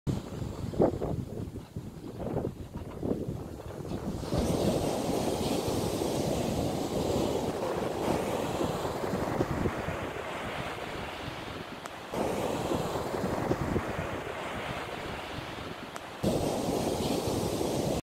Purple Jellyfish spotted at kerala beach
Crores of Jellyfish spotted at trivandrum kerala beach. around 4km sea shore spotted purple Jellyfish. this may be due to turbulence in the sea.